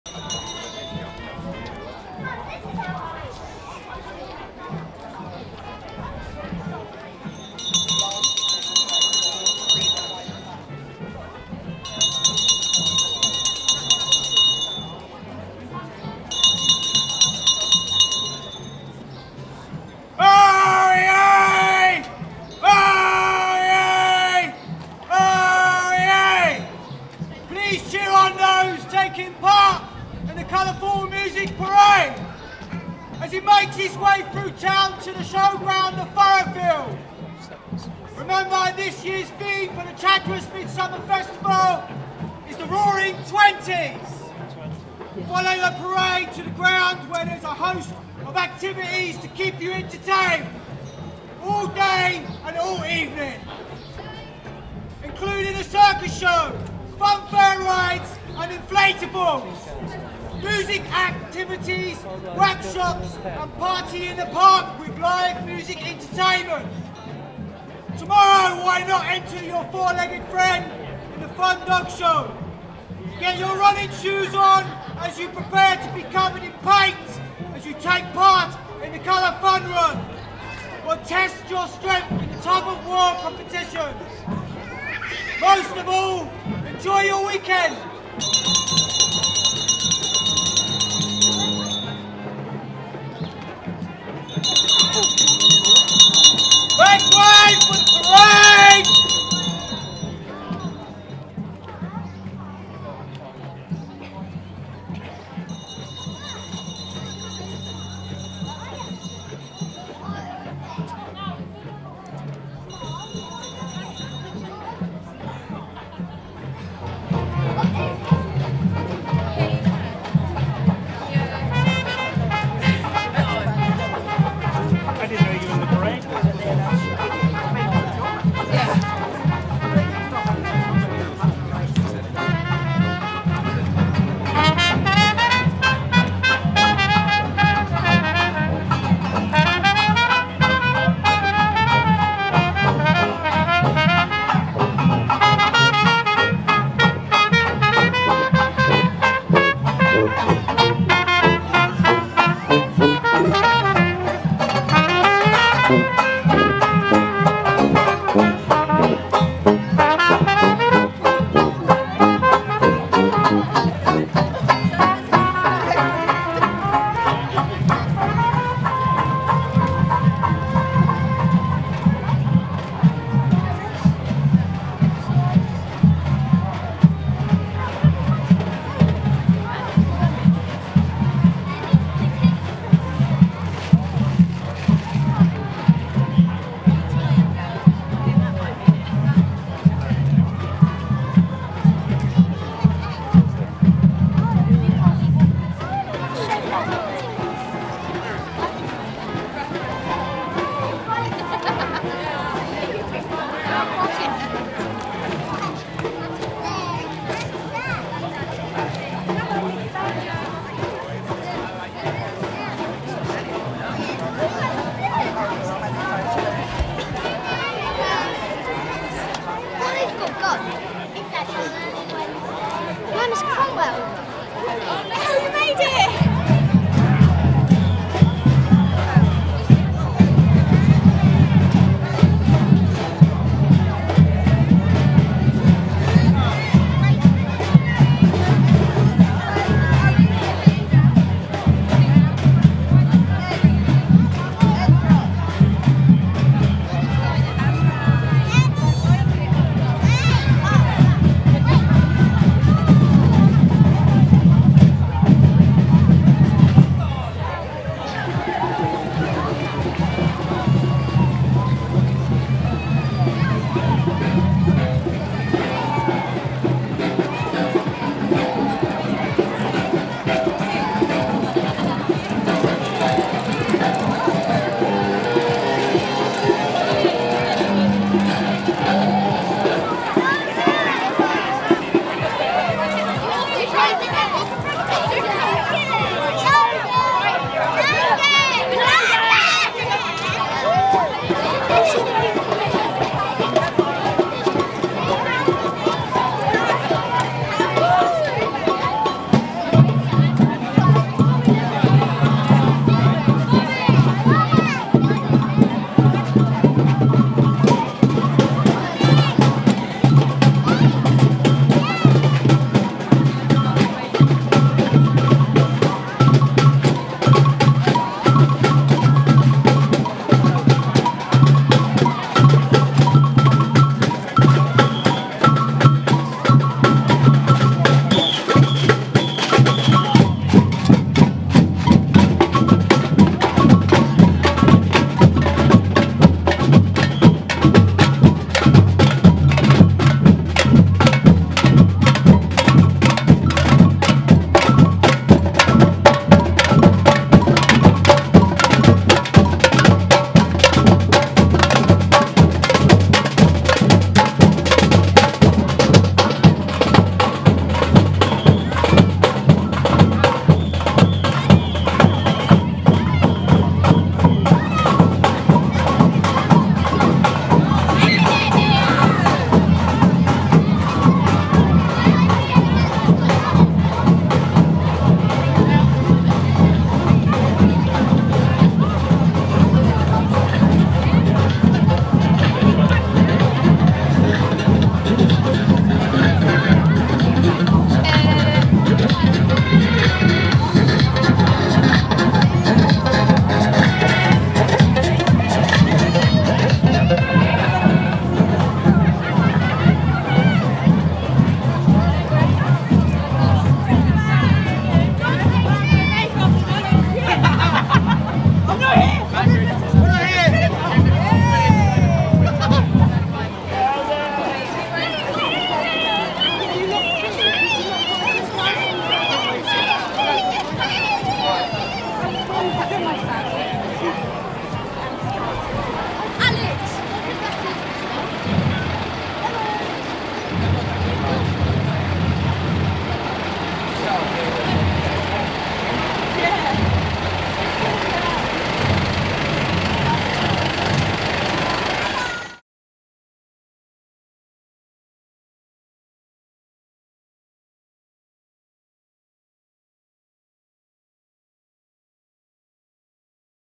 Chatteris Midsummer Festival Parade 2023
The Midsummer Festival parade in Chatteris, Cambridgeshire
24 Jun 2023